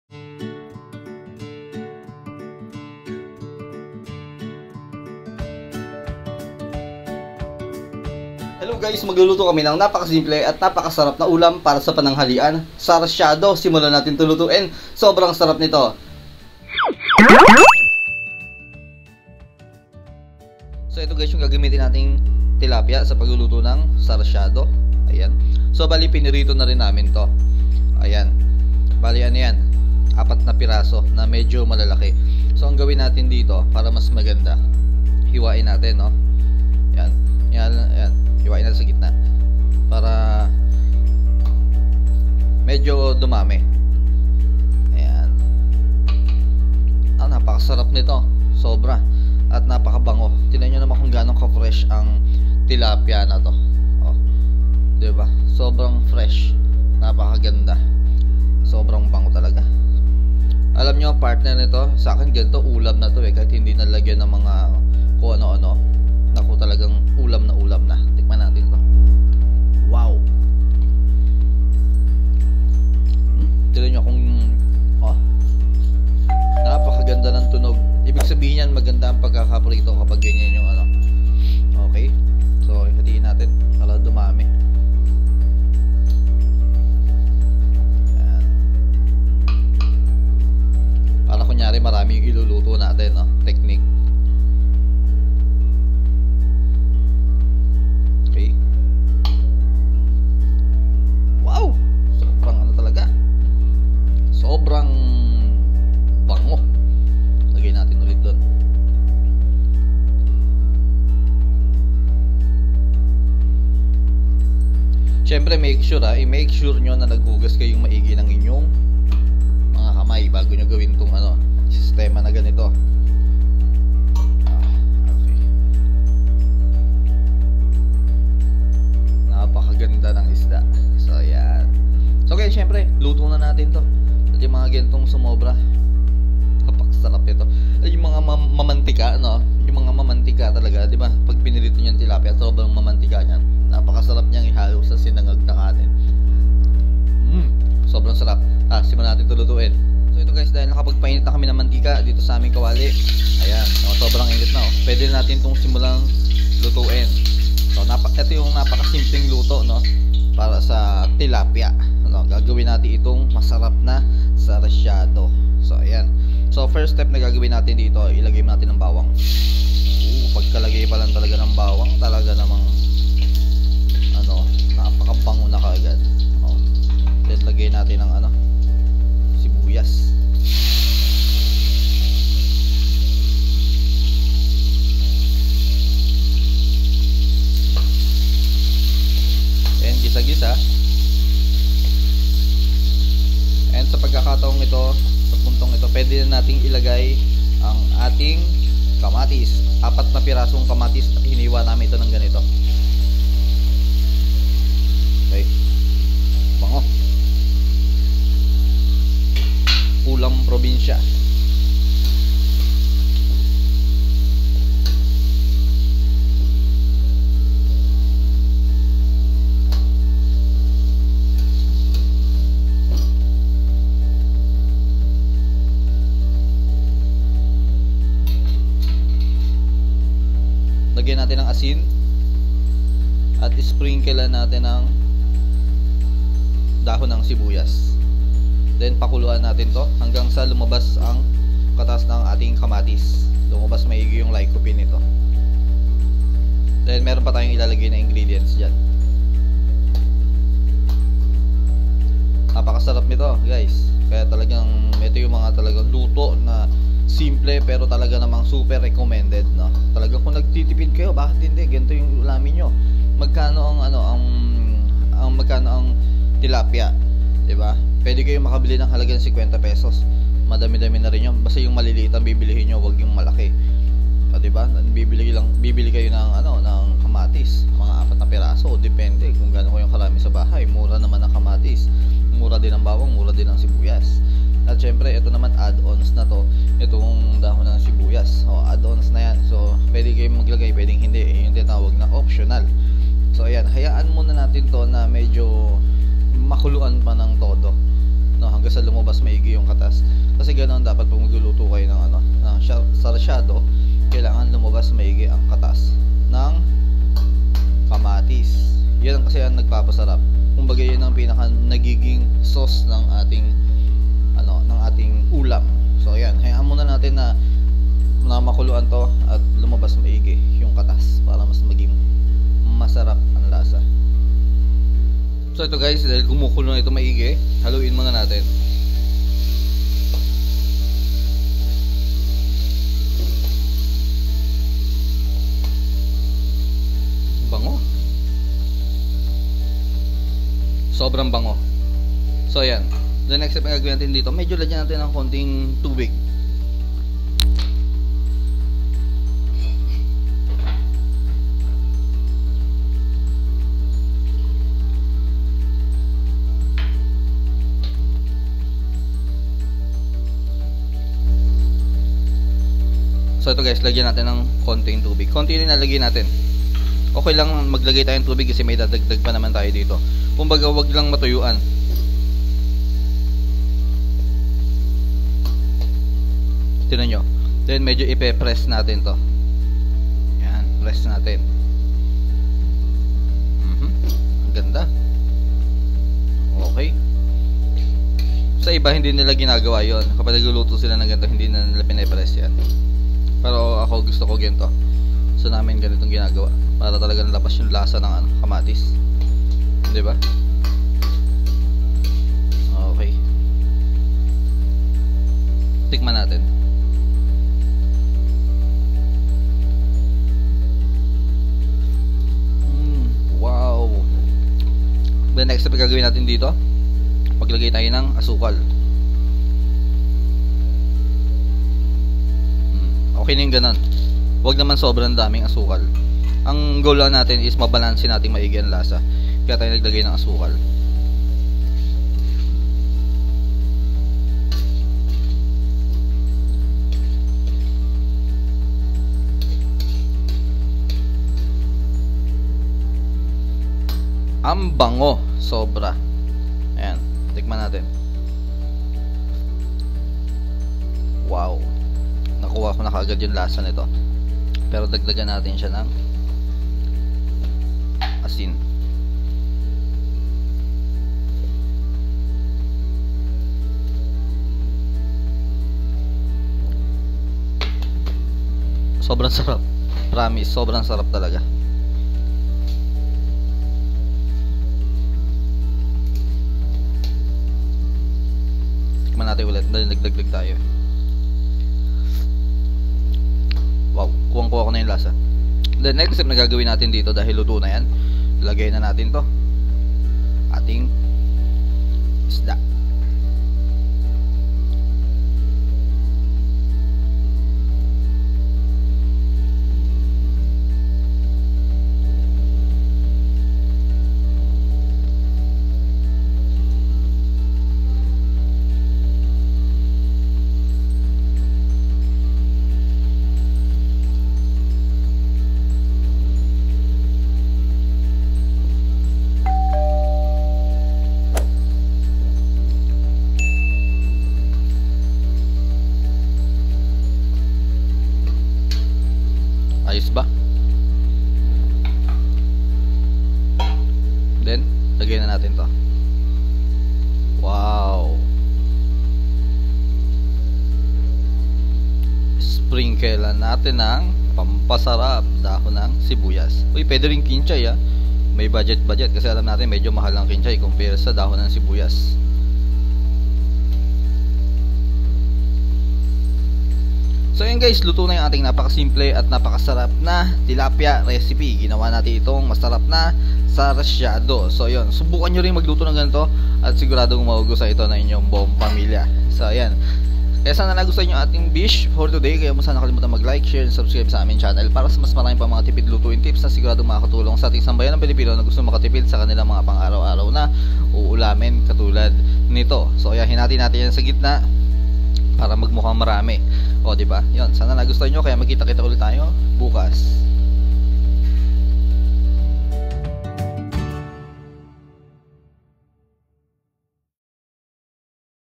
Akala ko hindi sila nasarapan sa luto ko pero ang dami nilang nakain sarciadong tilapia
nagluto kami ng npakasimple at napakasarap na ulam para sa pananghalian sarciadong tilapia napakasarap lalo na kapag niluto nyong maigi ang kamatis talagang napaka lasa at nilagyan namin ito ng dahon ng sibuyas para lalo itong maging mas masarap pwede din na kinchay ang ilagay
subukan nyo din na magluto ng ganito at siguradong magugustuhan ito ng inyong buong pamilya